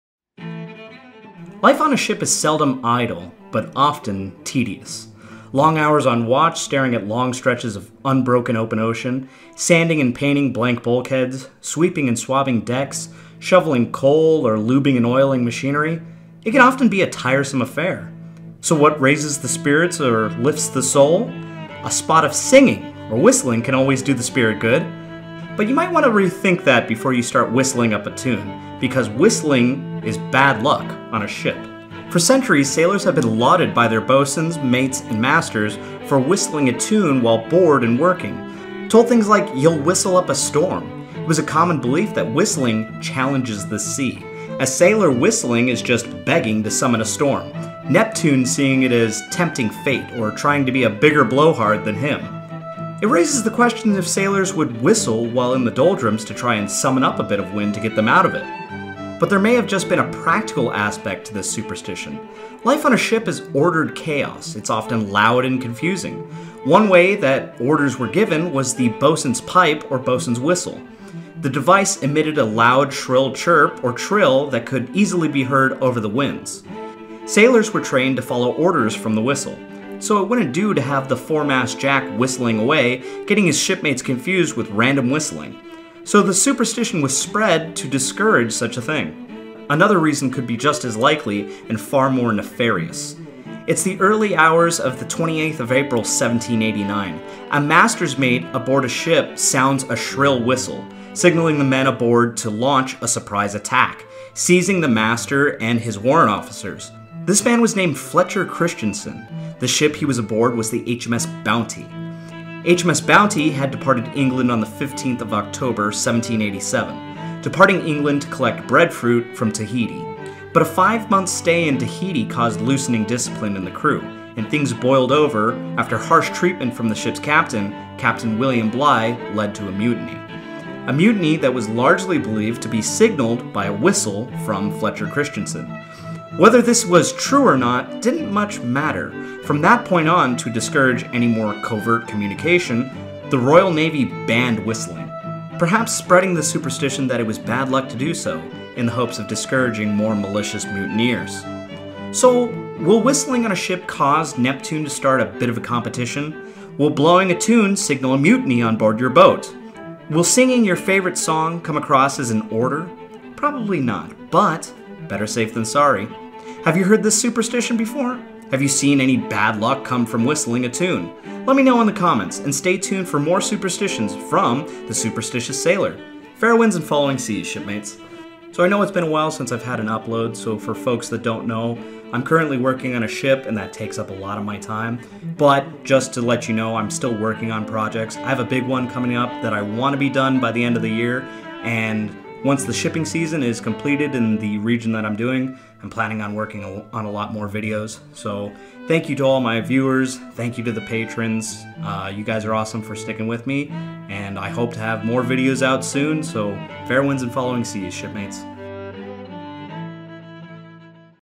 The Superstitious Sailor: Whistling on a Ship
"You'll whistle up a storm", how true is that statement, and where did it come from. Find out here!